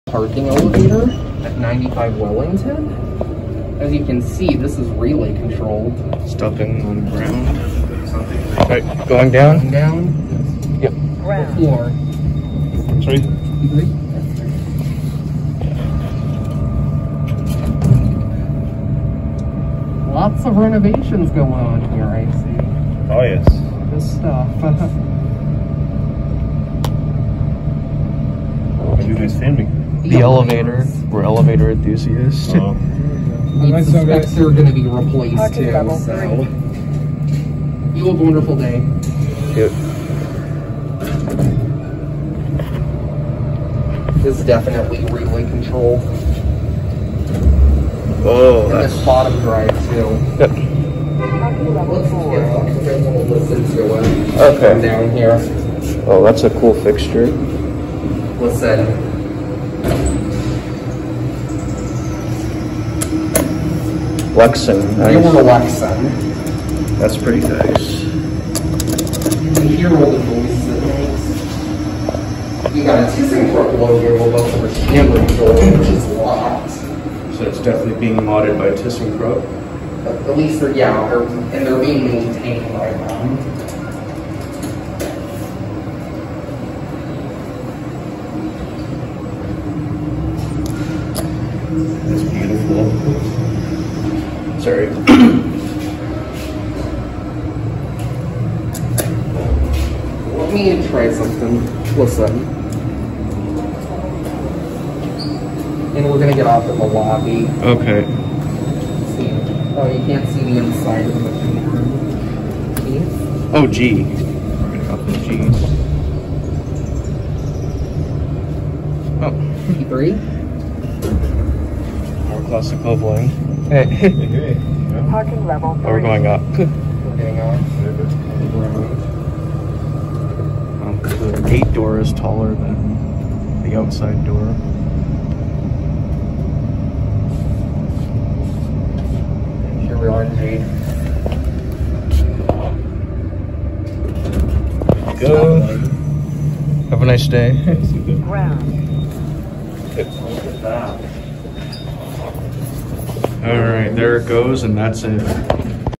Dovis Traction Elevator @ 95 Wellington St. W., Toronto, ON W/ Floridian Elevators
Recorded April 22nd, 2022. Epic classic levelling on this. This is about to get modded by TKE (probably in the process now as this was filmed 4 months ago).